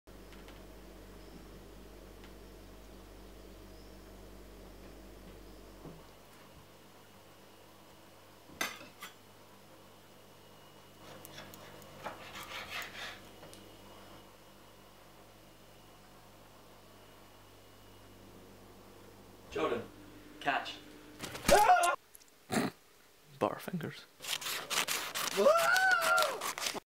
Catch! (The Throwing Knife)
Campbell makes a sandwich.

This was shot in at least 20 minutes. It's our first attempt at a throwing knife.